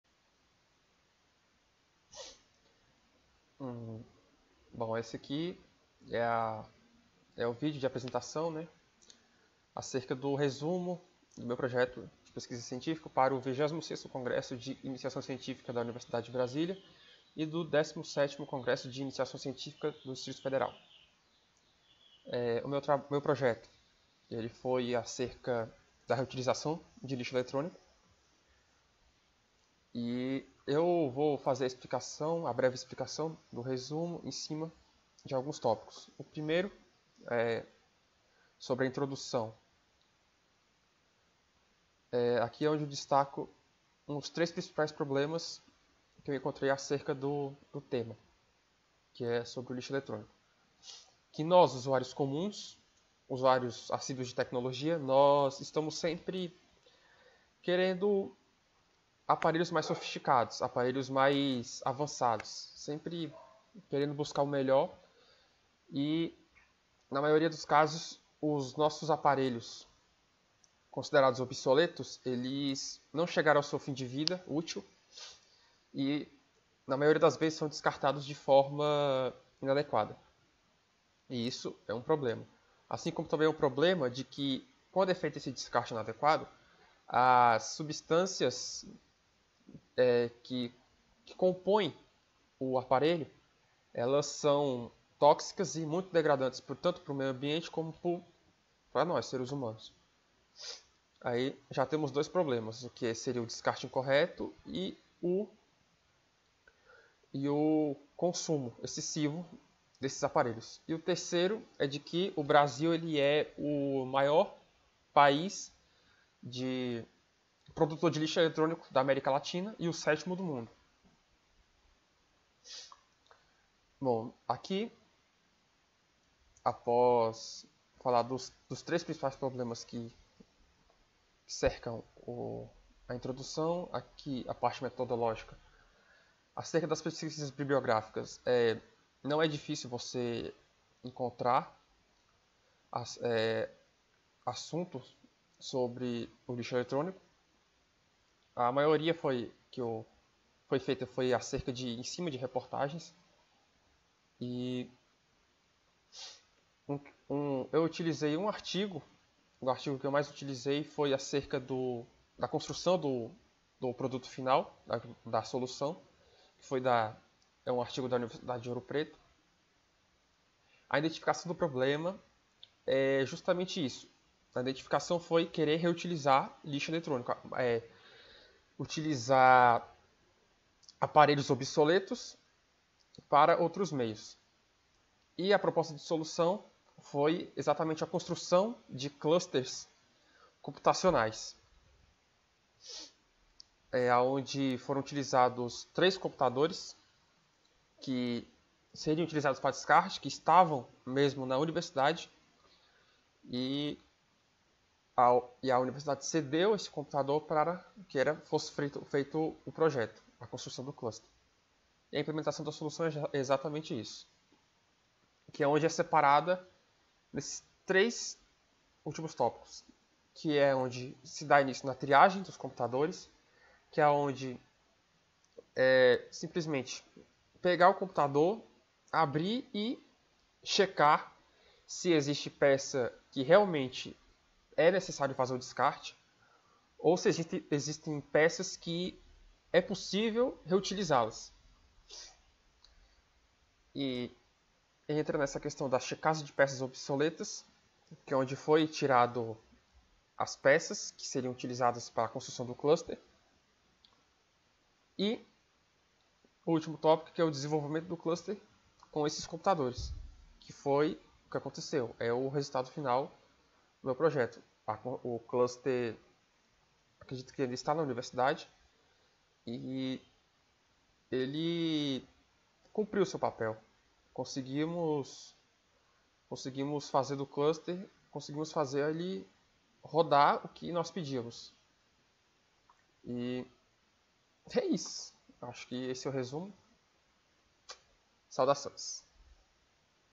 (147-UCB) Reutilização de lixo eletrônico
"Autor: Thomas Carvalho de Araújo
Orientador: Graziela Ferreira Guarda
Colaboradores:
Palavras-Chaves: Reutilização, cluster, lixo eletrônico."